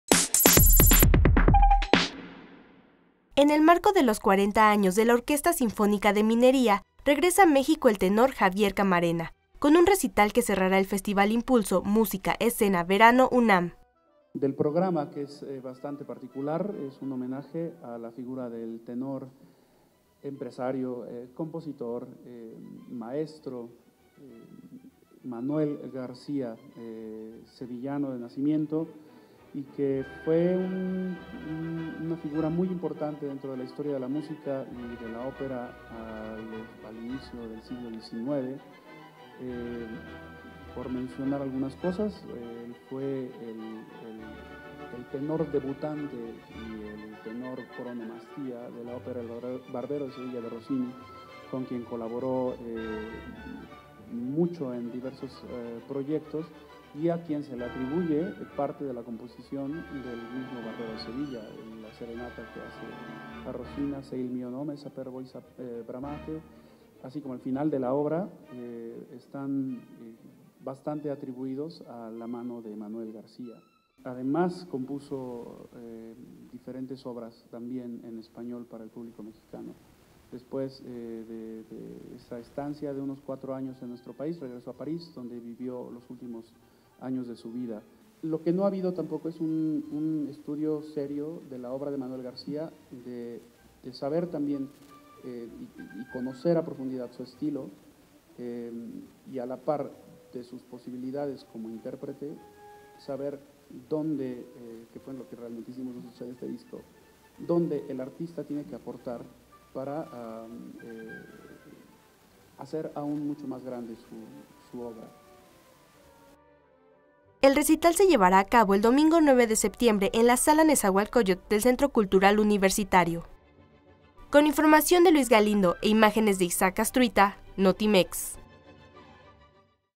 El tenor Javier Camarena regresa a México
En el marco de los 40 años de la Orquesta Sinfónica de Minería, regresa a México, el  tenor Javier Camarena, con un  recital que cerrará el festival IM•PULSO Música Escena Verano UNAM.